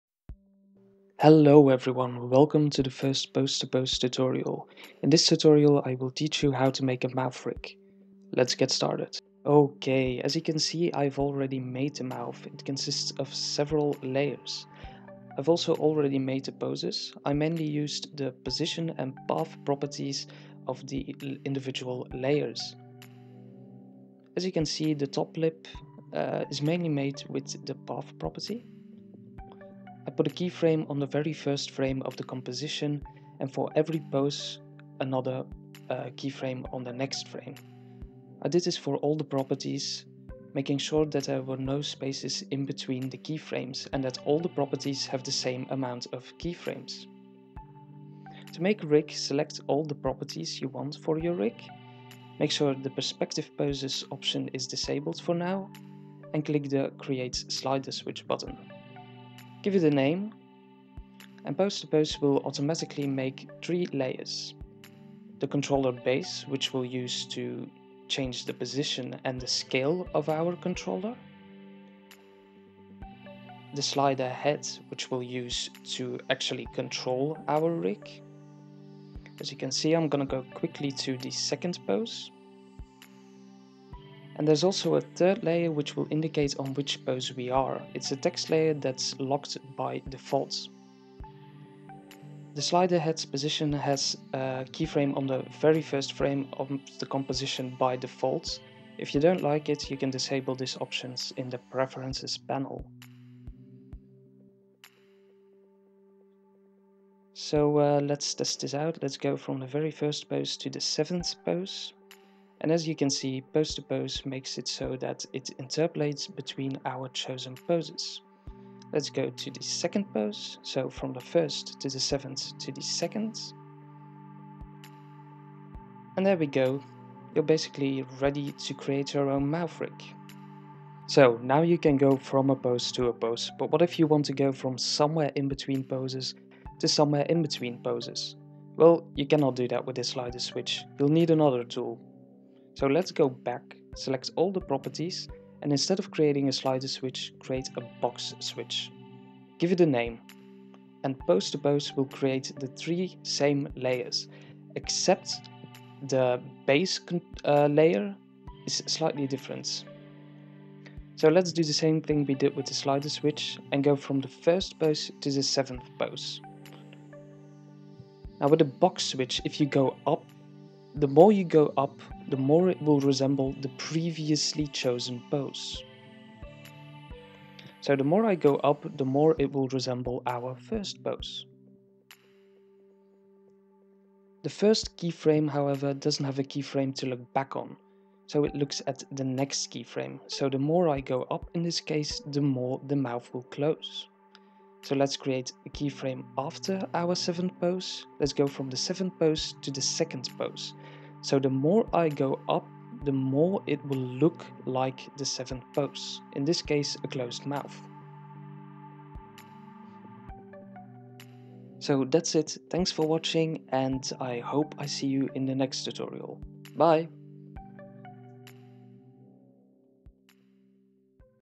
After Effects Mouth Rig - Pose2Pose Tutorial
In this tutorial I will teach you how to use Pose2Pose to make a mouth rig using the Slider & Box Switches

Pose2Pose is an facial rigging system. It's the best script for rigging mouths, eyes, eyebrows and so much more. Thanks to the included joystick you are also able to create 2.5D head rigs. While the advanced 'Perspective Poses' option gives your rigs a new dimension.